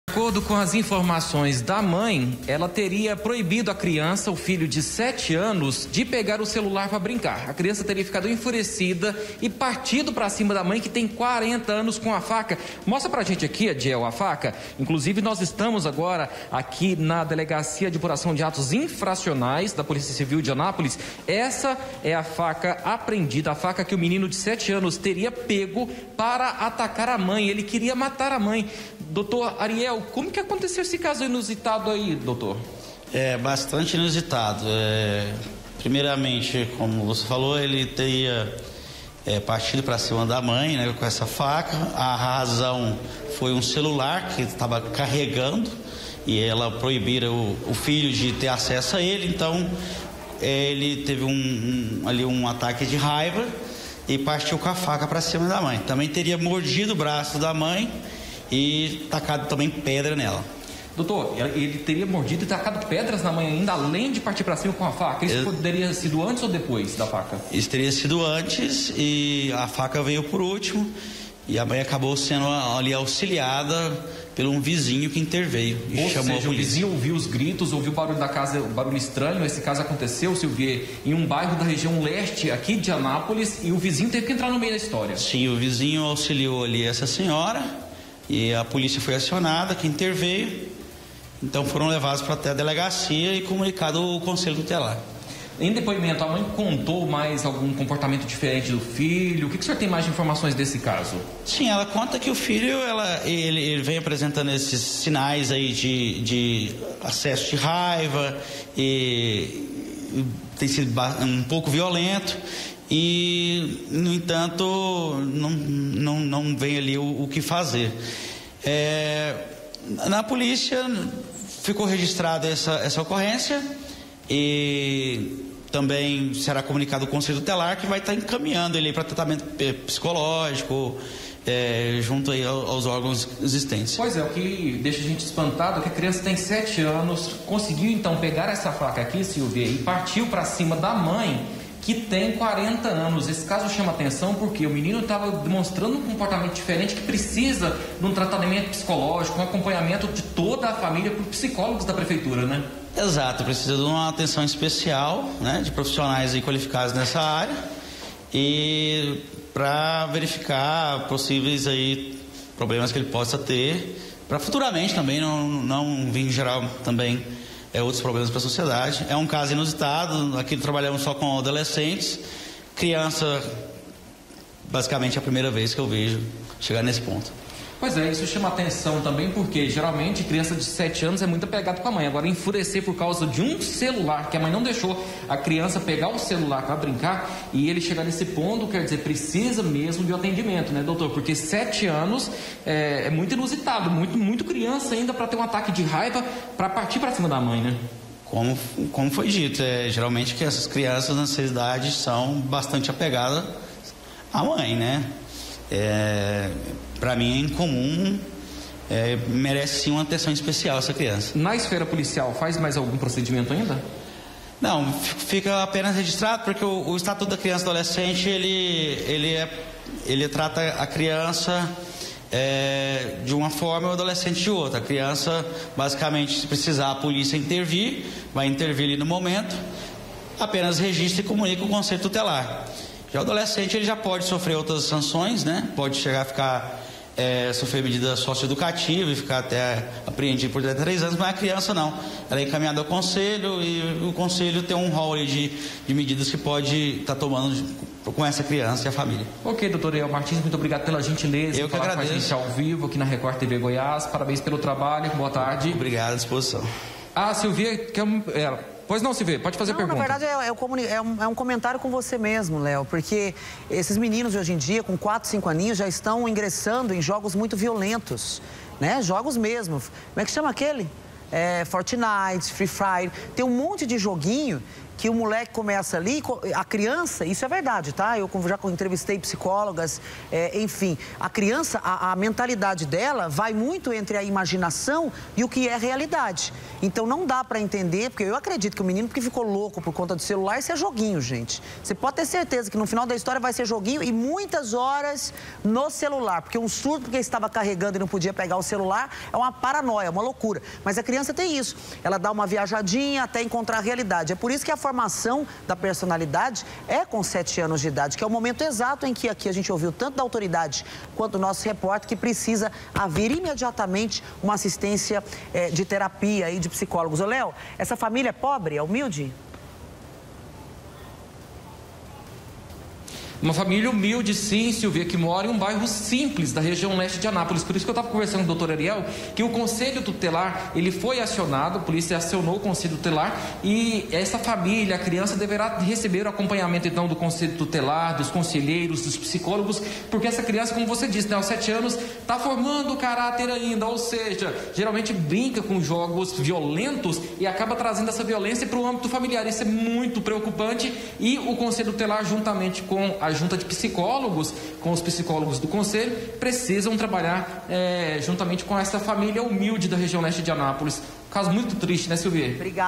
ANÁPOLIS: MENINO DE 7 ANOS TENTA MATAR A MÃE DEVIDO CELULAR
UM MENINO DE APENAS 7 ANOS TENTOU MATAR A PRÓPRIA MÃE COM UMA FACA. ISSO FOI EM ANÁPOLIS E FOI A PRÓPRIA MÃE DA CRIANÇA QUE RELATOU O CASO À POLÍCIA. EU VOLTO COM O LEONARDO GONÇALVES. ELE ESTÁ COM O DELEGADO QUE APURA O CASO E TEM MAIS INFORMAÇÕES PRA GENTE.